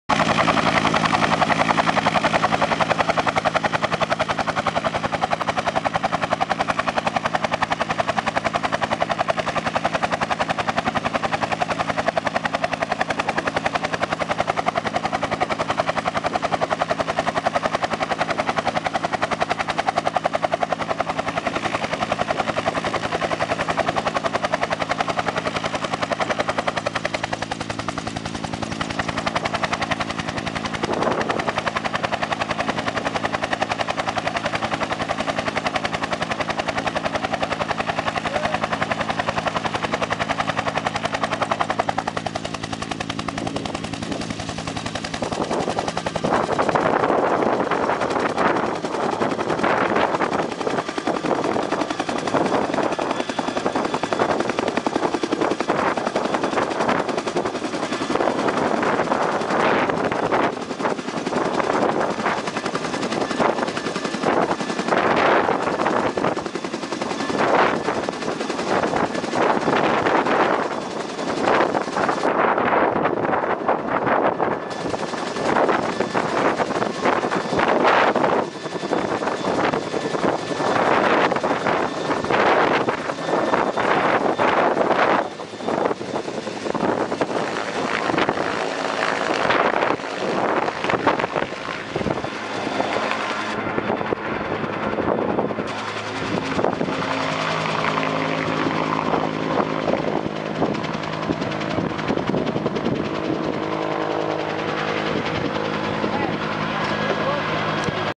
Trout Stocking on the Lower Saluda River
After driving over 150 miles and taking a helicopter ride thousands of trout are now swimming in the Lower Saluda River.
Congaree Riverkeeper was happy to be able to participate in this project with DNR and SLED.